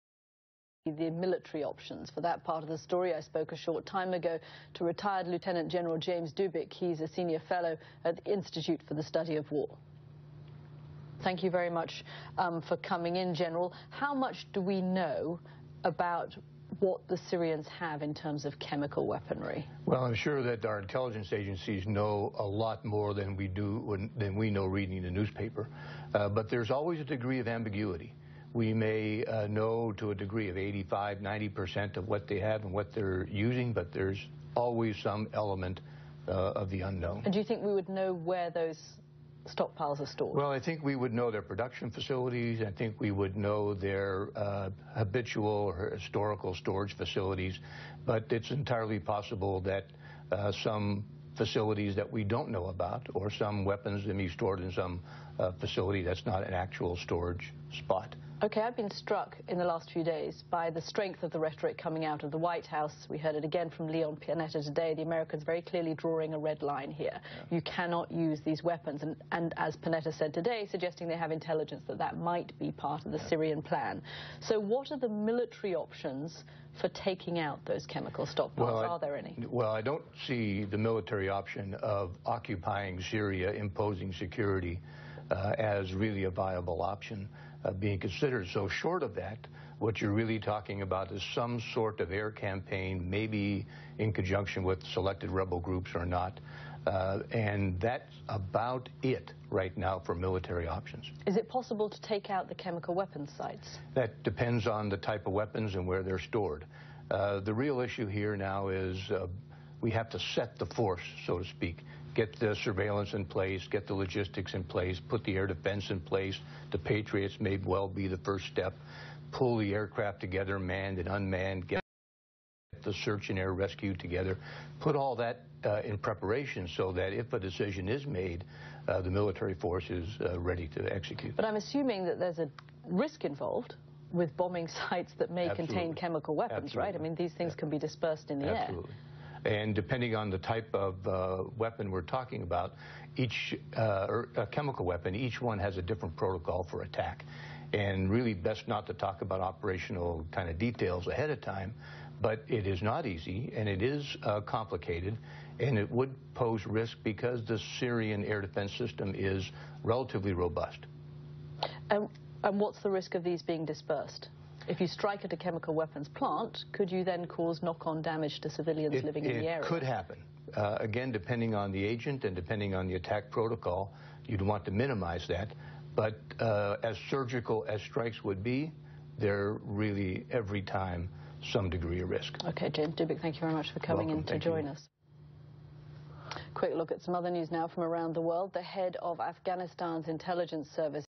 LTG James M. Dubik Discusses Developments in Syria (BBC)
ISW Senior Fellow Lieutenant General James M. Dubik (U.S. Army, Ret.) discusses recent developments in Syria, including possible regime use of chemical weapons.